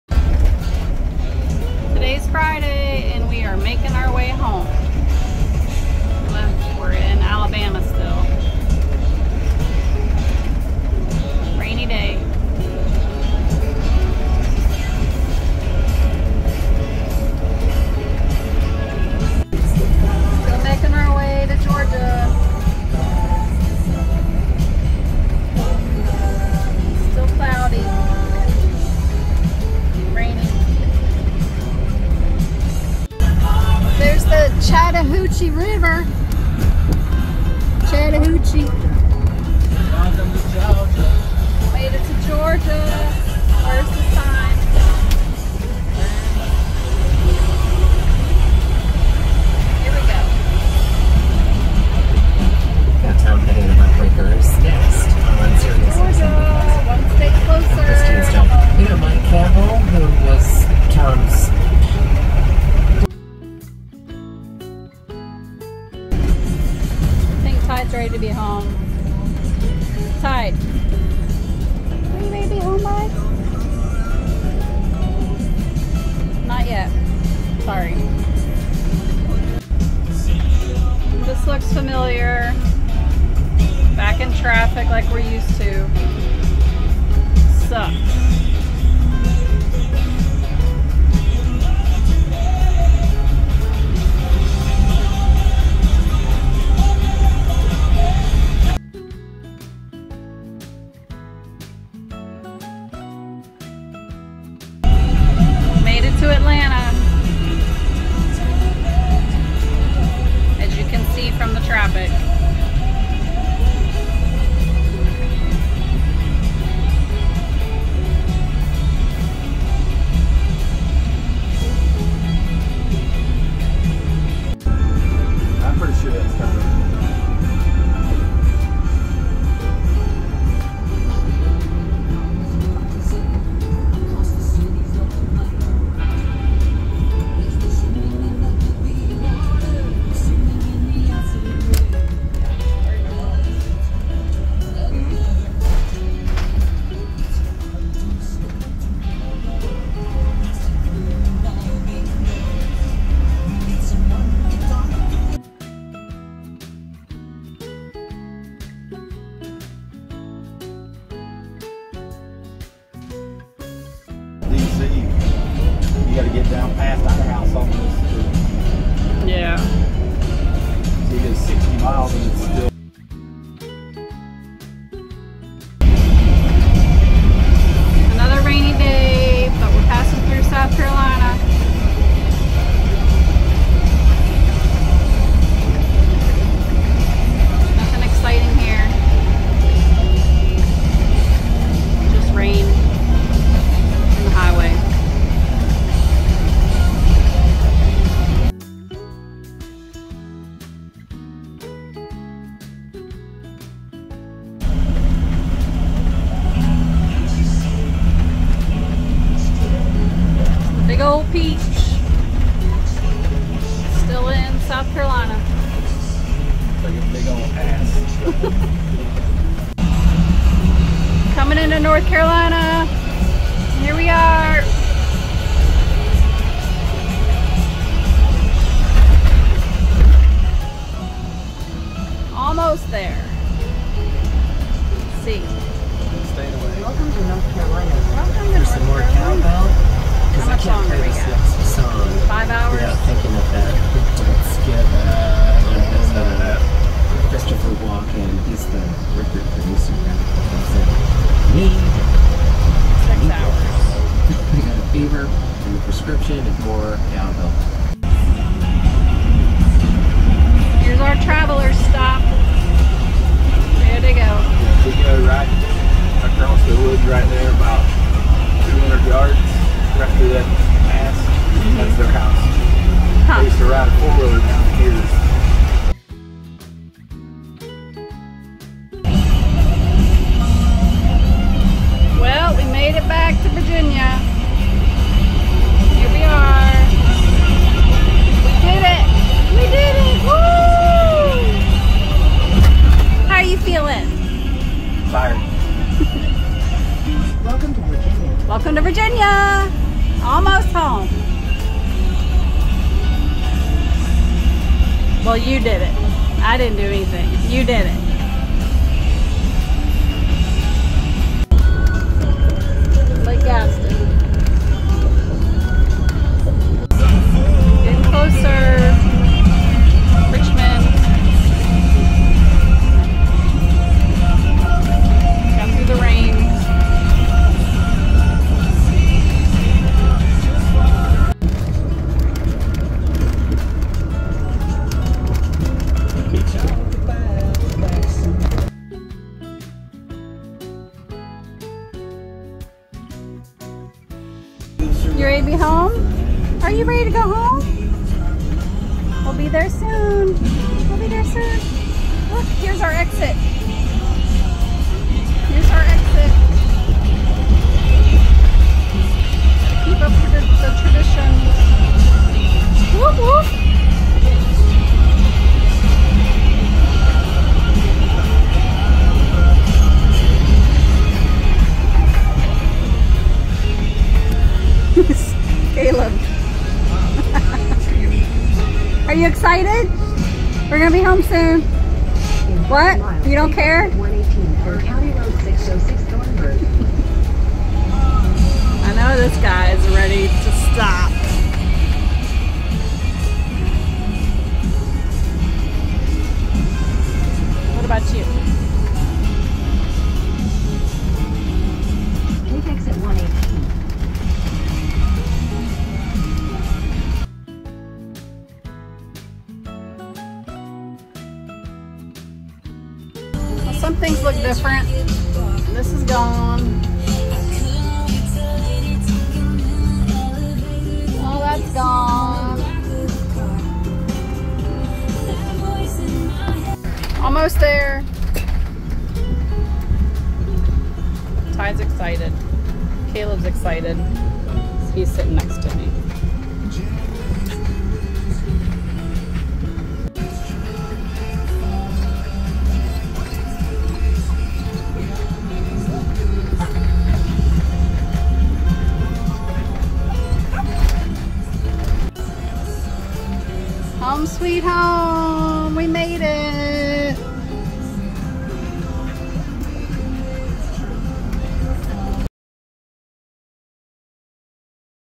Day 54 Home bound!
Drove through Alabama, Georgia, SC, and NC, to Home sweet home VA ❣️.  We had a rainy drive both days but Jason pushed through to get us home in 2 days.  Loved the experience and all of the beautiful places we visited making memories that we'll treasure for a lifetime!  Jason is the BOMB for driving EVERYDAY on this trip.